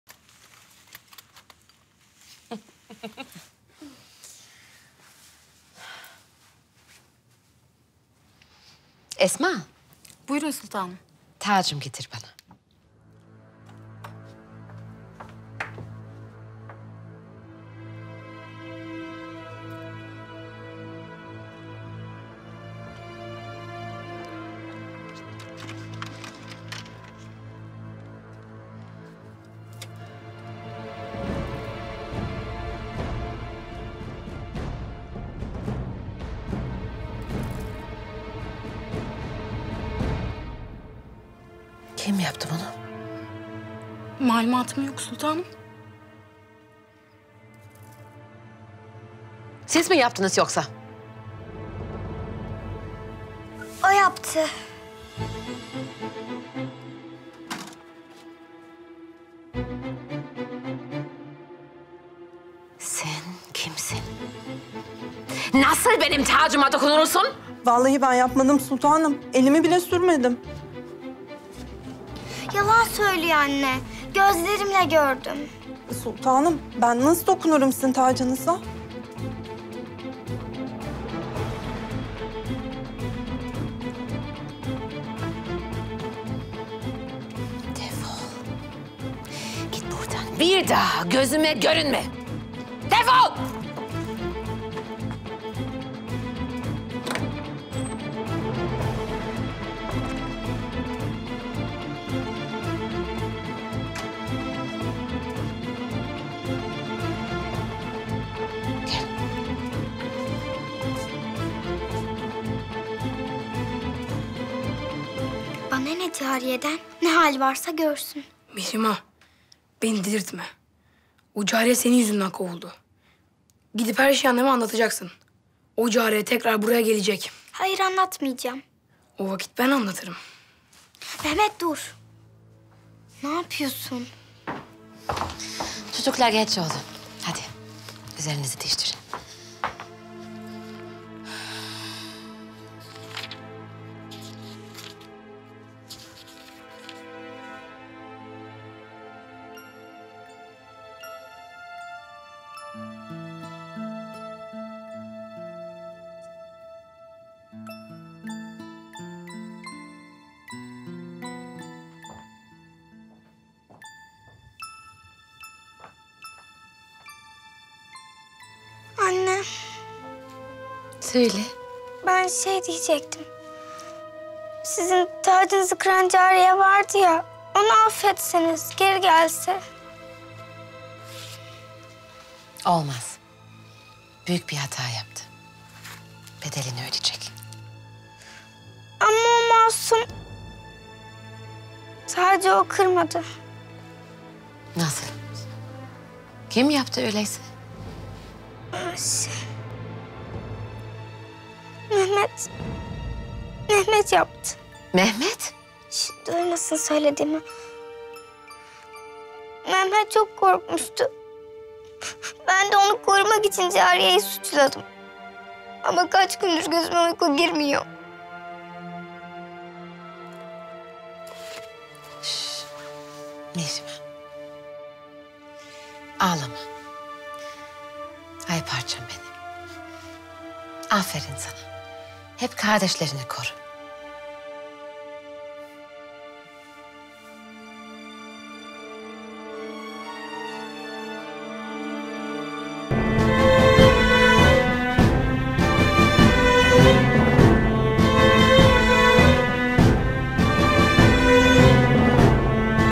O Gün Annemin Kızı Olacağım Belliydi 😈
Validemin tacına göz dikip kırdıysam ve bunu Mehmet'in üstüne attıysam ne olmuş yani?

Ben Mihrimah. Cihan padişahı Sultan Süleyman Han’dan olma, Hürrem Sultan’dan doğma... Asaletini ve gücünü babasının kanından, cürretini ve zekasını annesinden alan... Güneşin ve ayın sultanıyım.